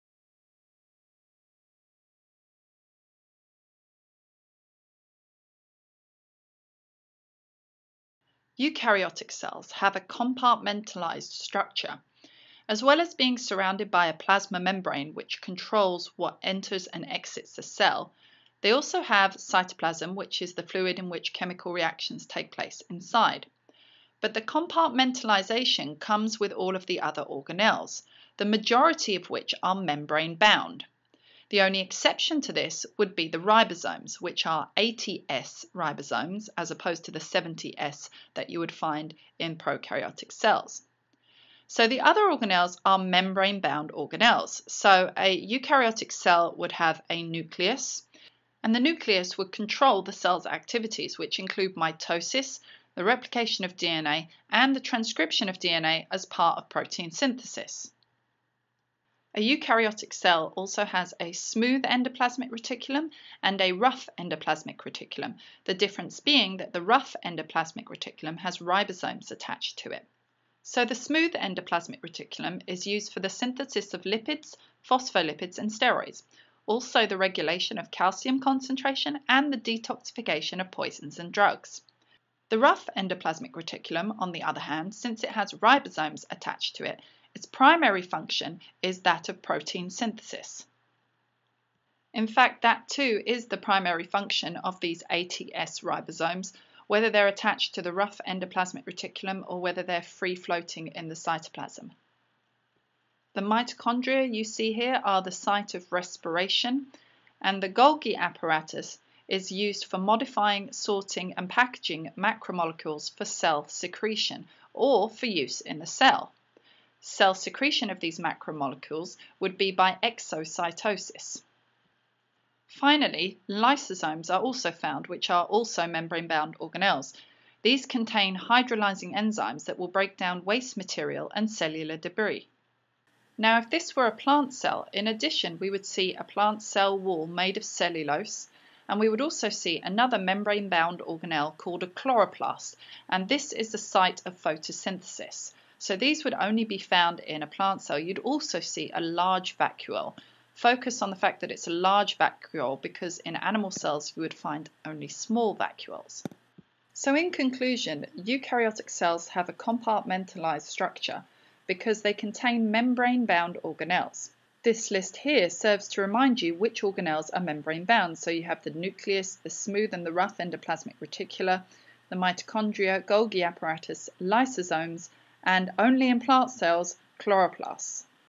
1.2 Eukaryotic cells have a compartmentalized structure
Describing the structure of eukaryotic cells and the function of their main organelles.